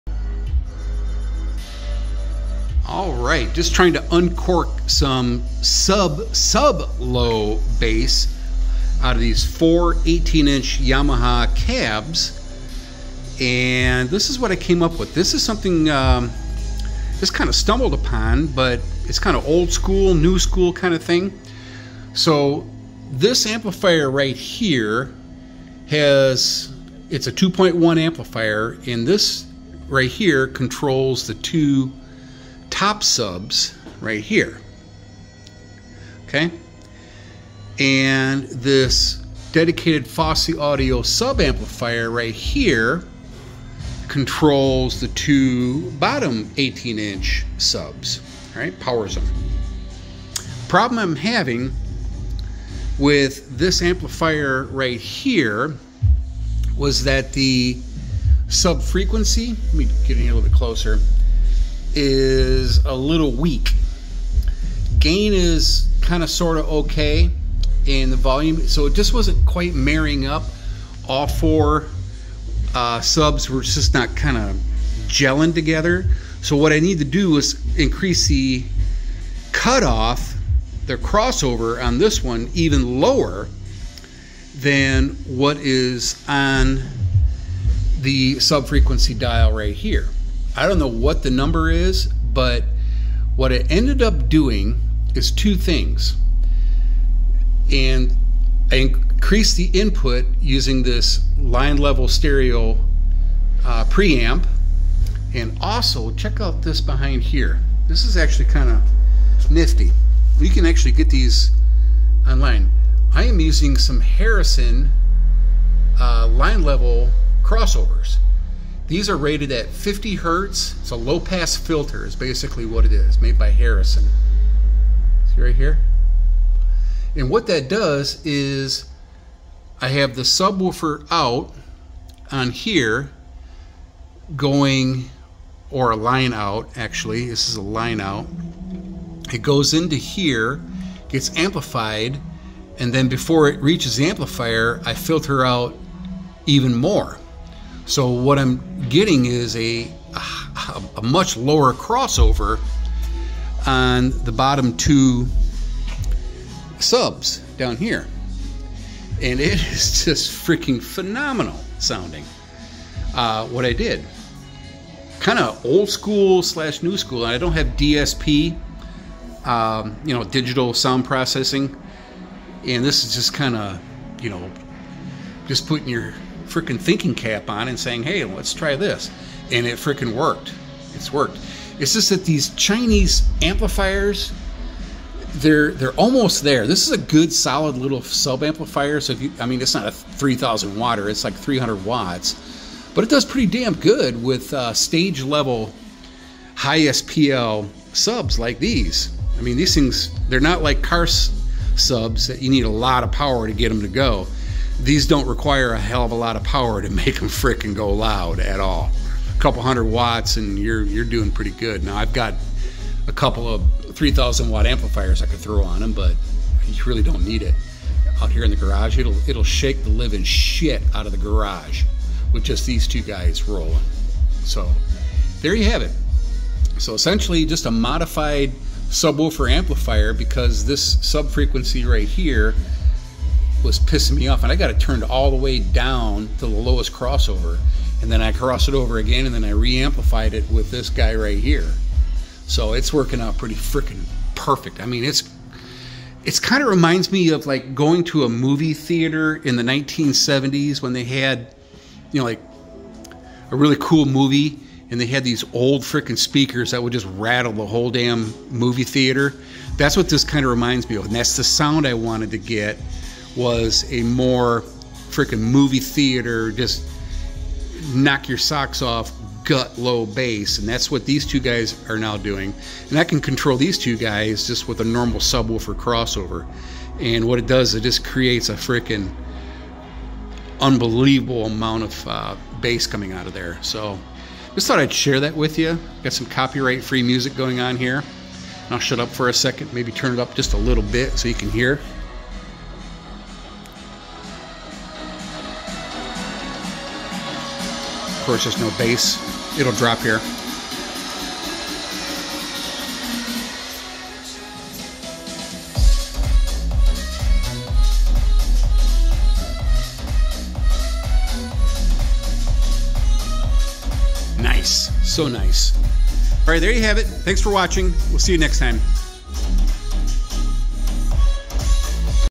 Subwoofer amplifier 50 Hz low pass "prefilter" + preamplifier mod = max low bass extension!
MUCH better low-low bass growl from these four 18" Yamaha subwoofer cabs. NOTE: the two small amplifiers come with a 24v (5-amp) power brick. I am using 32v (5-amp) for more power output. With these high SPL stage/studio/club speakers, you don't need a ton of power to get rowdy loud. I have a couple 3,000w amplifiers I could hook up when I want to get really stupid loud
Here are some links:
FMOD Harrison 50Hz line level low pass filter crossover
Stereo preamplifier (line level)
Fosi Audio M01 300w subwoofer amplifier
Fosi Audio BT30D Pro 2.1 Bluetooth amplifier

Chris DIYer Amazon Store: